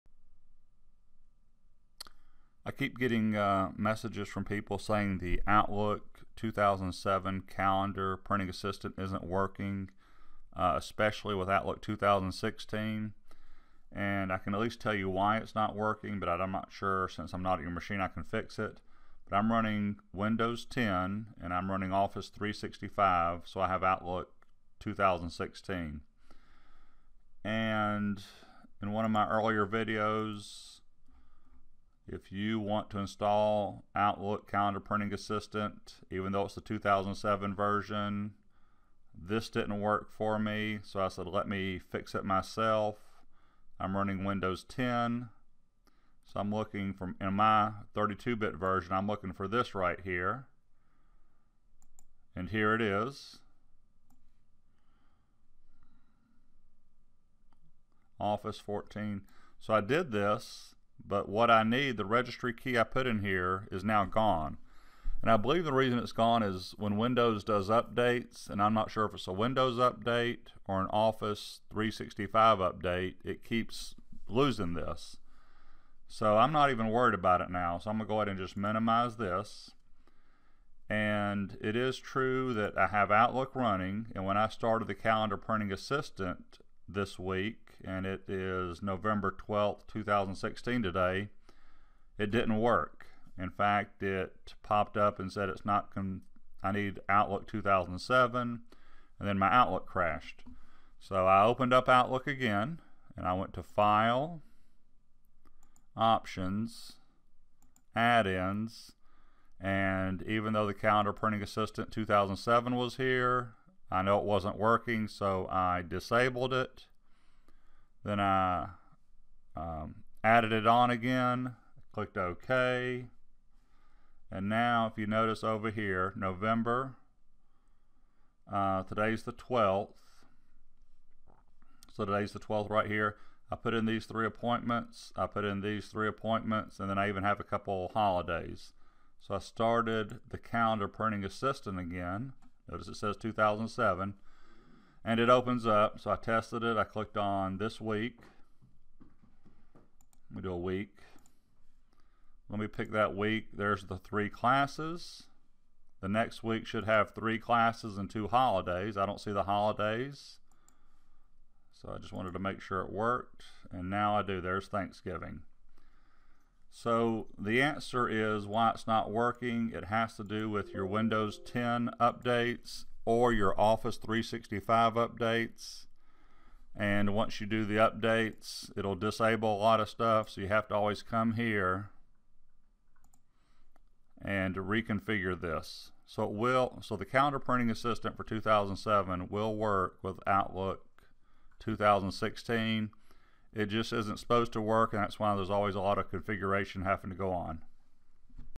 Outlook Calendar Priniting Assistant - 11/12/2016 - troubleshooting by Chris Menard
I made this video in November 2016. Windows updates and Office updates make this really difficult to use and I have to keep making tweaks. Technically it is not supposed to work with Outlook 2016/2013 anyway.

Windows updates or Office 365 updates are keeping the calendar printing assistant (CPA) 2007 from working with Outlook 2016. Even after I had it working for months, mine wasn't working today (11/12/2016).

I had to remove the CPA add-on in Outlook 2016, and then add it back. I had to do it twice for some reason.

I'm running:
1) Windows 10
2) Office 365 which means I'm using Outlook 2016

Date of this video: November 12, 2016

Note: The Calendar Printing Assistant for Outlook 2007 is not made to run with Outlook 2016. I've got it working, but it I do have to tweak it when Windows 10 updates. It is a pain to tweak, but I need to print multiple calendars every few months.

-- EQUIPMENT USED
○ My camera